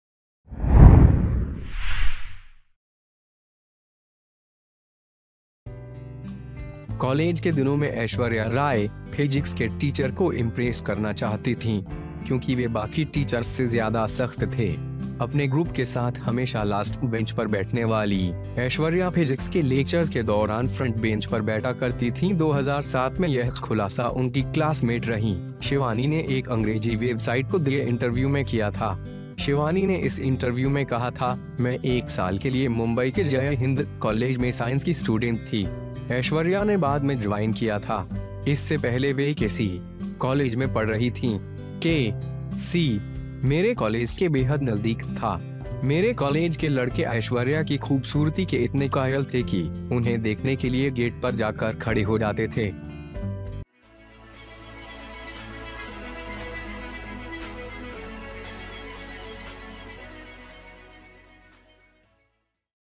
फिजिक्स टीचर को इम्प्रेस करना चाहती थीं ऐश्वर्या, क्लासमेट ने खोले थे राज
Aishwarya Rai wanted to impress the physics teacher in college days. Because they were more strict than the rest of the teachers. Aishwarya, who always sat on the last bench with her group, sat on the front band during the lectures of Physics. In 2007 it was revealed that Shivani, who was in his classmate, did an interview with an English website. What did Shivani say ...

Shivani said in this interview, "I was a science student of Jai Hind College in Mumbai for a year, Aishwarya had joined later, before she was studying at KC College."
- "KC was very close to my college. The boys of my college were so convinced of the beauty of Aishwarya that they would stand on the gate to see them.

HJ NEWS !!